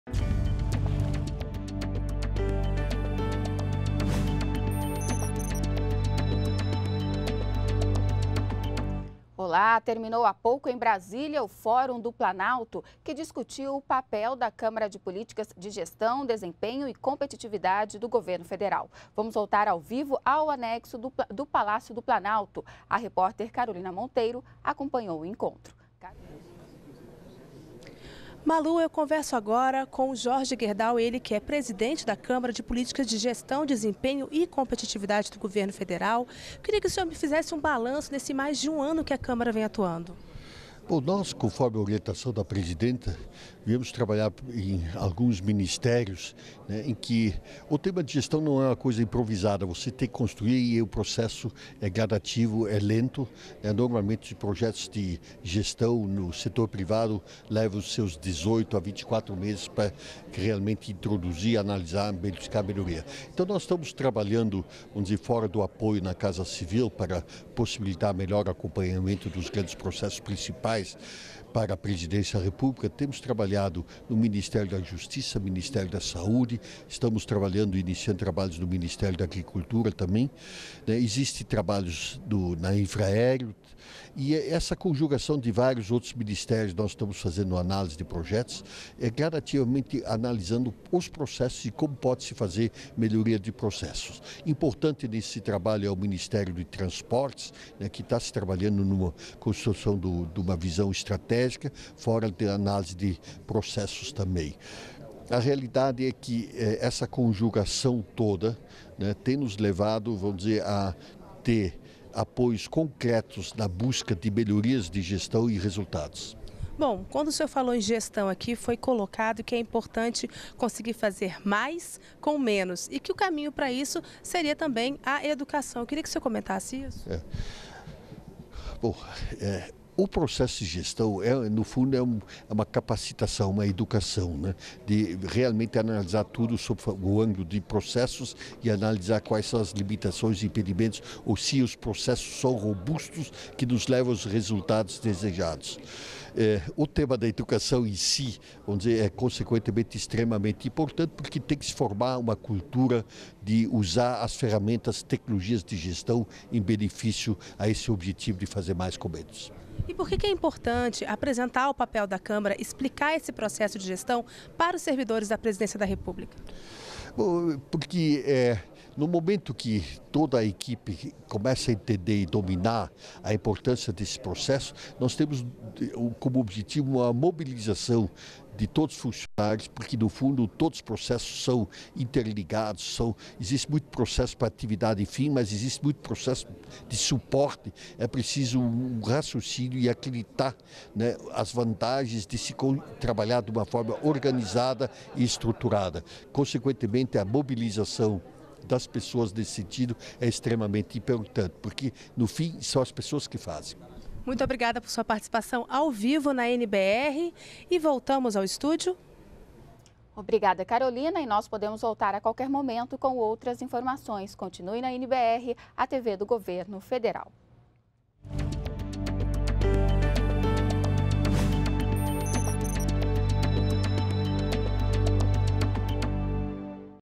Empresário Jorge Gerdau reforça atuação da Câmara de Políticas de Gestão e Competitividade
ATIVIDADES DO GOVERNO - 23.08.12: O governo federal realizou nesta quinta-feira (23), em Brasília, o Fórum do Planalto. O tema do debate foi o papel da Câmara de Políticas de Gestão, Desempenho e Competitividade, que tem o objetivo de de formular medidas para melhorar a gestão pública. A  repórter Carolina Monteiro, conversou com o presidente da Câmara de Gestão e Competitividade, Jorge Gerdau, e traz mais detalhes.